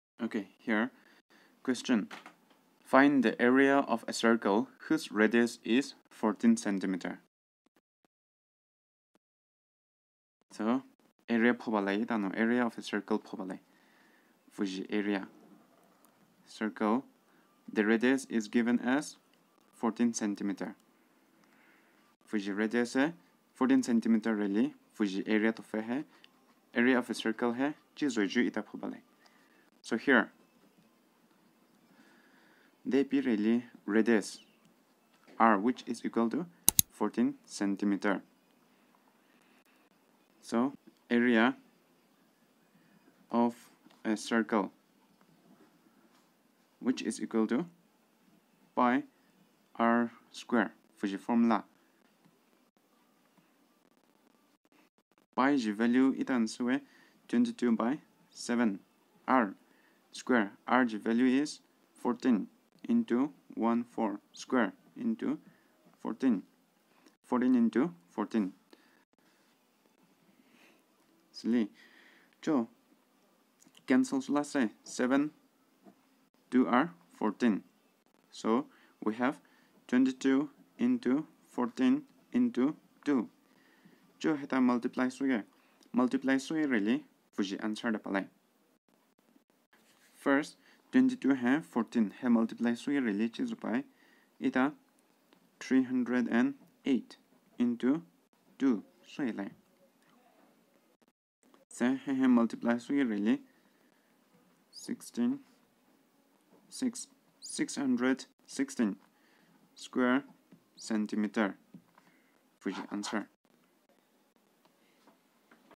Ex. 12.1 Question 1 Find the area of a circle whose radius is 14 cm Class 10 Mensuration
UPI ID: - elokhokhrasi@oksbi